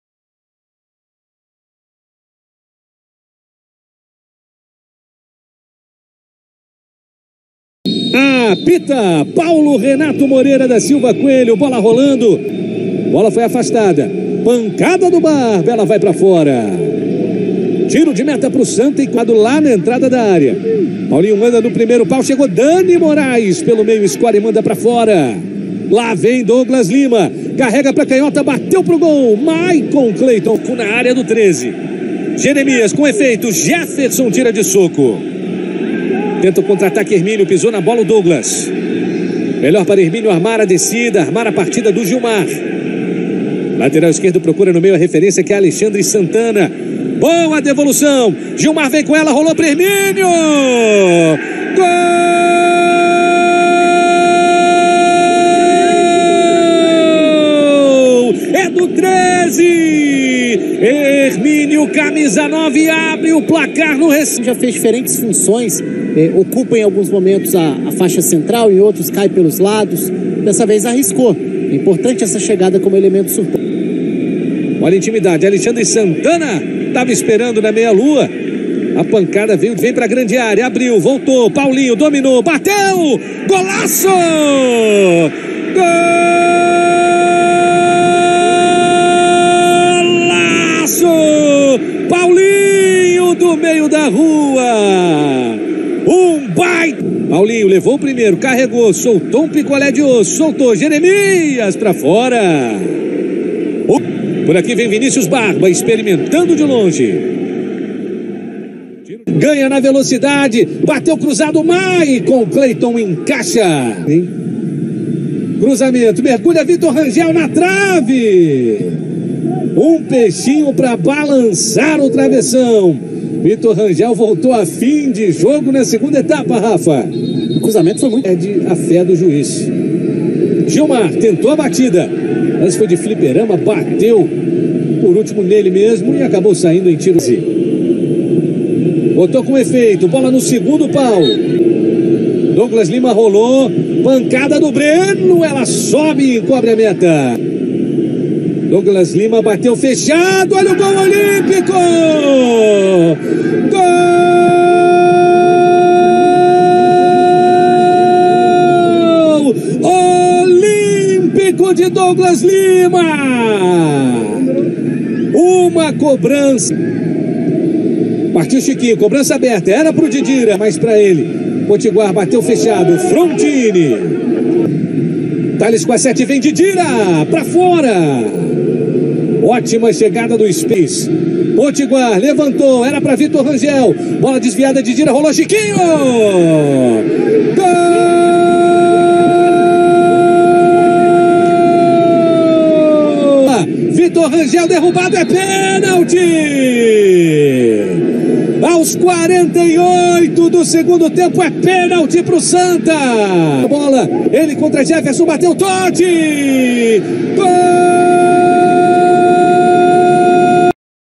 Gol Olímpico e virada nos acréscimos, Santa Cruz bate Treze pela Série C; veja os gols
A partida se encaminhava para vitória do Treze. Porém, no futebol, tudo pode acontecer. De forma heróica, o Santa empatou aos 46 minutos com Chiquinho. Já aos 49, veio a virada Tricolor no Arruda após o árbitro assinalar uma penalidade para os donos da casa. Na cobrança, Tote venceu o goleiro Jefferson e decretou números finais na partida.